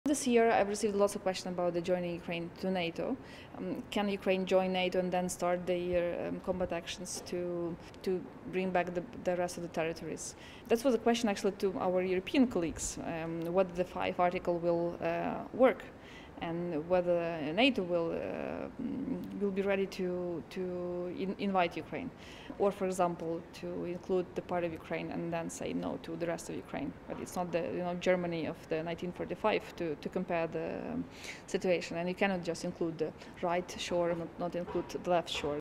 WSF 2023 - Solomiia Bobrovska
Solomiia Bobrovska - Member of Parliament, Committee on National Security & Defence, Deputy Head of the Ukrainian Delegation in NATO PA, Ukraine.

Interview during Warsaw Security Forum 2023.